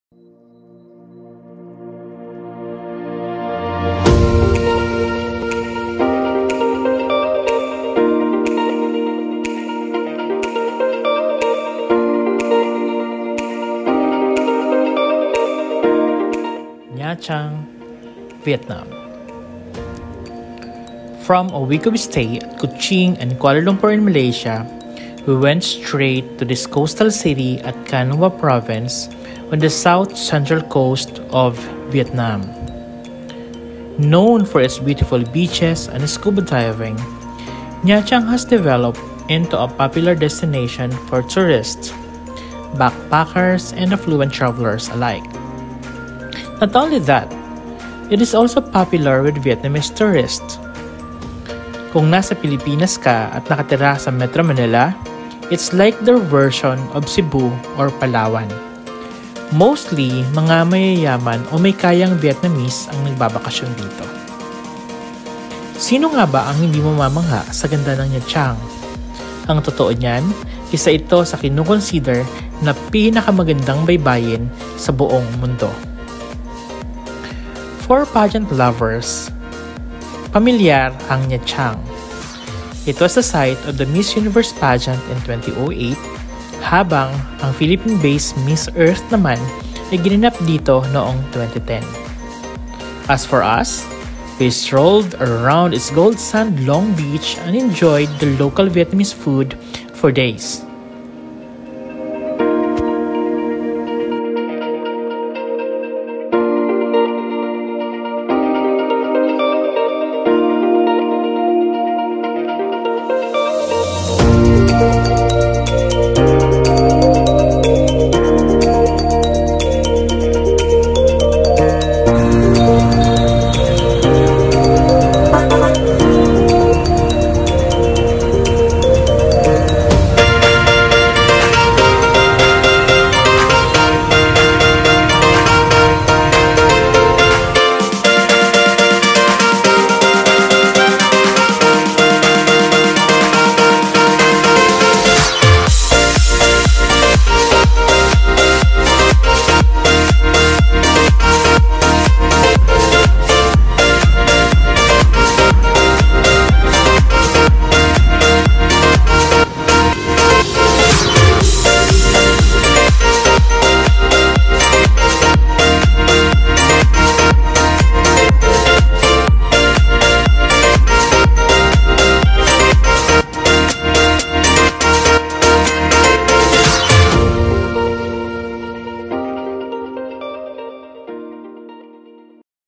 NA THRANG: This Vietnam's Town Homes The World's Best Beach
From a week of stay at Kuching and Kuala Lumpur in Malaysia, we went straight to this coastal city at Khánh Hòa Province, on the South Central Coast of Vietnam.

Known for its beautiful beaches and scuba diving, Nha Trang has developed into a popular destination for tourists — backpackers and affluent travelers alike. Not only that, it is also popular with Vietnamese tourists.

For Filipinos living in Metro Manila, it’s like their version of Cebu or Palawan. Mostly, middle-class or rich Vietnamese visit here for holidays. Who wouldn’t be fascinated by its beauty? In fact, it is widely considered as among the world's most beautiful bays.

Nha Trang is familiar for pageant lovers: it was the site of the Miss Universe pageant in 2008; while the Philippine-based Miss Earth held its 2010 edition of the pageant here.As for us? We strolled around its gold-sand Long Beach, and enjoyed the local Vietnamese food for days.